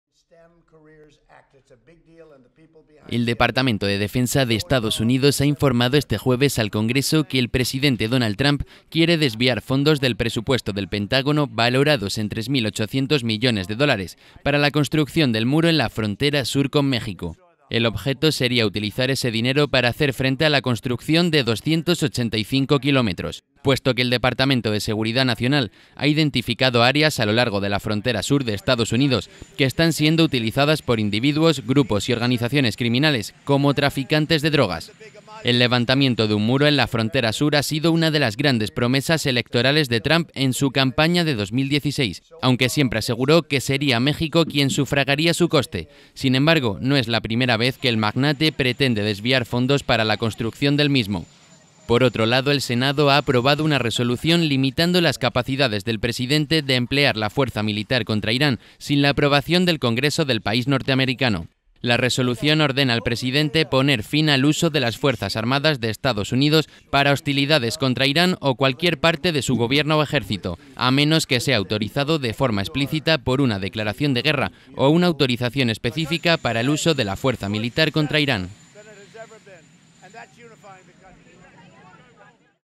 Trump quiere desviar más dinero para la construcción del muro en México
El departamento de Defensa de Estados Unidos ha informado este jueves al Congreso, que el presidente Donald Trump quiere desviar fondos del presupuesto del Pentágono, valorados en 3.800 millones de dólares, para la construcción del muro en la frontera sur con México.